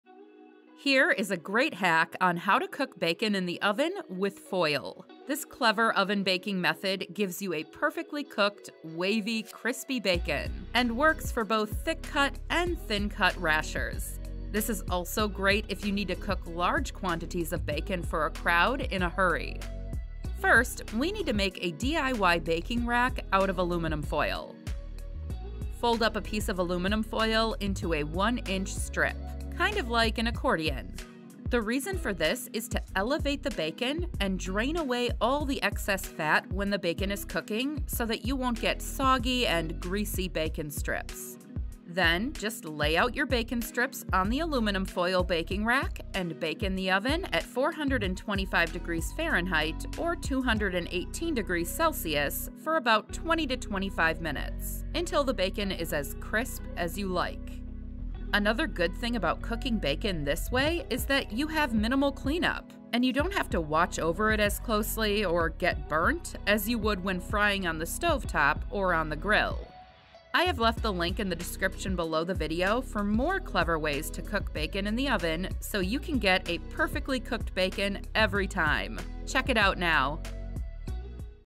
How To Cook Bacon In The Oven With Aluminum Foil (Cleanest And Healthiest Cooking Method)
Here is a great hack on how to cook bacon in the oven with aluminum foil. This clever oven-baking method gives you a perfectly cooked, wavy, crispy bacon and works for both thick-cut and thin-cut rashers. This is also great if you need to cook large quantities of bacon for a crowd in a hurry.

➡️ How To Cook Large Bacon In The Oven (3 Easy Methods)

How to cook bacon in the oven with aluminum foil?

First, we need to make a DIY baking rack out of aluminum foil.

Fold up a piece of aluminum foil into a one-inch stripe, kind of like an accordion. The reason for this is to elevate the bacon and drain away all the excess fat when the bacon is cooking so that you won't get soggy and greasy bacon stripes.

Then, just lay out your bacon strips on the aluminum foil baking rack and bake in the oven at 425°F or 218°C for about 20-25 minutes until the bacon is as crisp as you like.

Another good thing about cooking bacon this way is that you have a minimal cleanup, and you don't have to watch over it as closely to get your bacon burnt as you would when frying on the stovetop or on the grill.

I have left the link at the beginning of this description for more cleanest and healthiest ways to cook bacon in the oven so you can get perfectly cooked bacon every time.